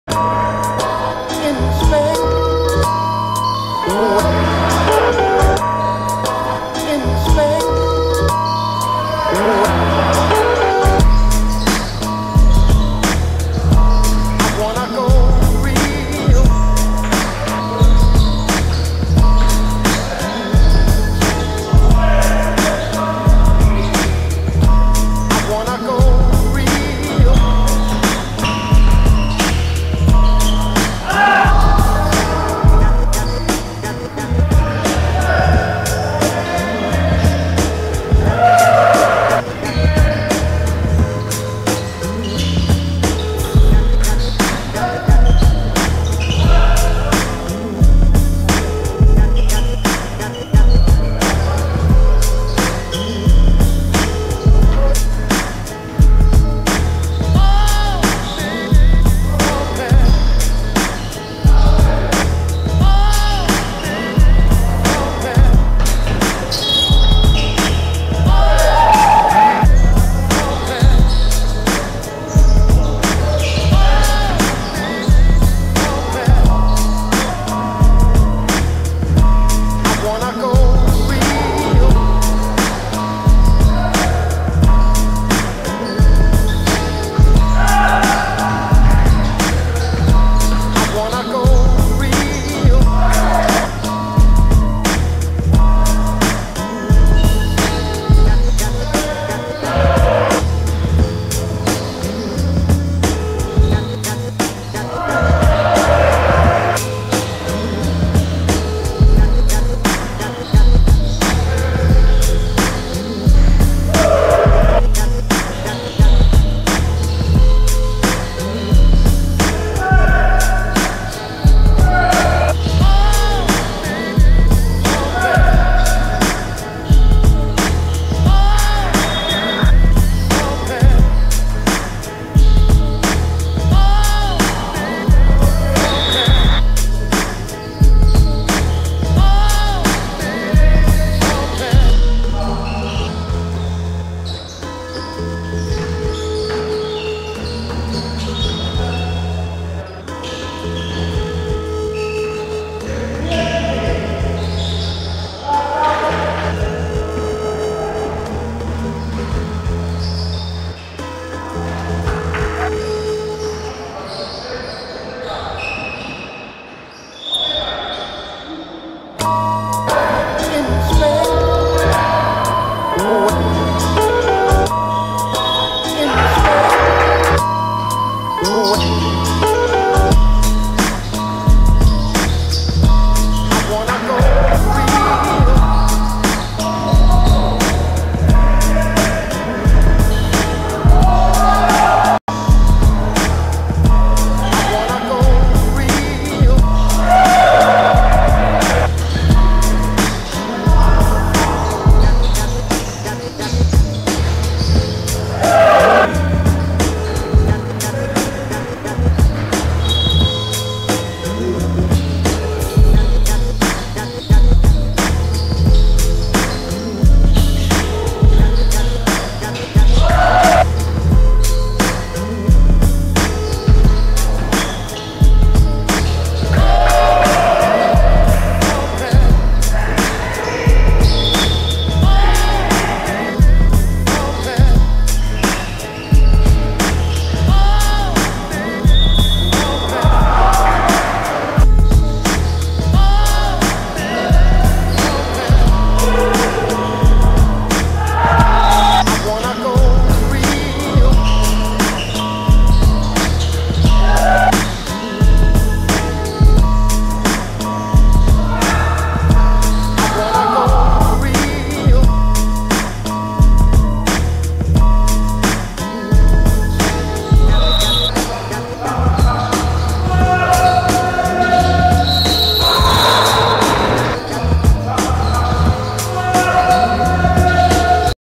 21 10 Houston vs Miami – Basket 36+ Ochota
HOUSTON 48:50 MIAMI
W.Jabłoński 26pkt 3ast
L.Łukomski 12pkt 7zb 3ast 3stl